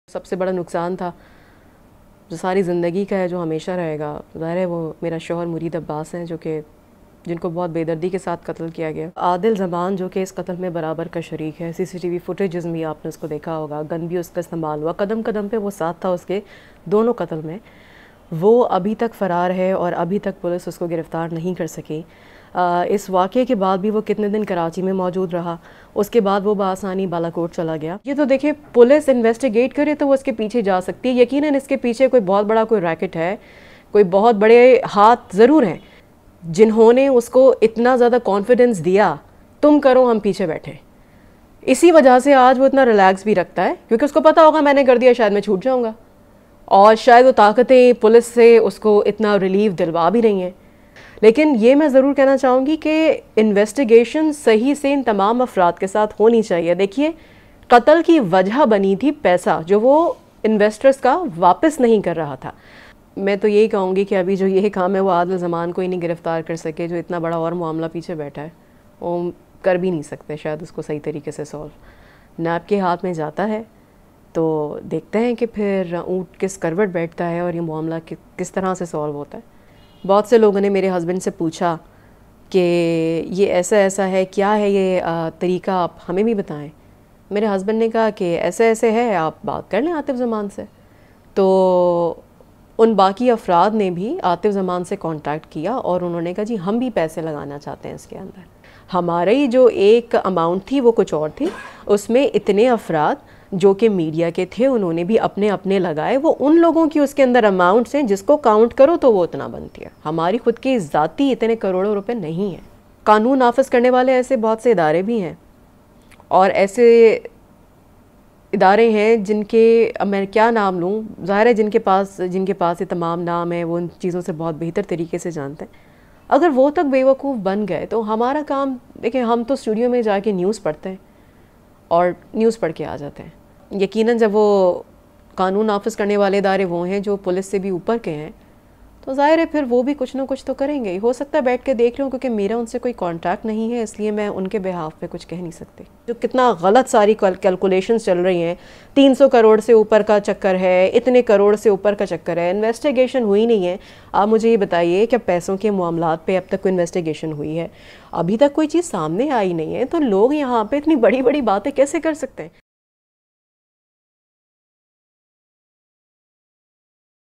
Mureed Abbas' Murder Case: 'There's definitely a big racket behind this' - BBCURDU
کراچی میں قتل ہونے والے ٹی وی اینکر مرید عباس کی بیوہ زارا عباس نے کہا ہے کہ پیسہ ہی ان کے شوہر کے قتل کی وجہ بنا تاہم یہ رقم کتنی تھی اس بارے میں غلط اعدادوشمار پھیلائے جا رہے ہیں۔ CLICK HERE to subscribe to BBC Urdu :